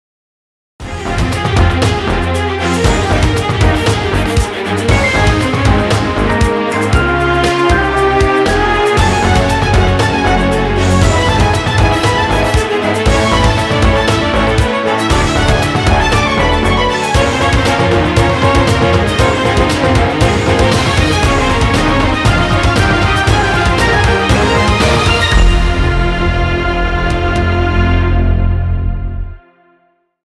Broadcast style of motion design
"Broadcast" one of the styles in the motion design
Programs:
After Effects, Premier pro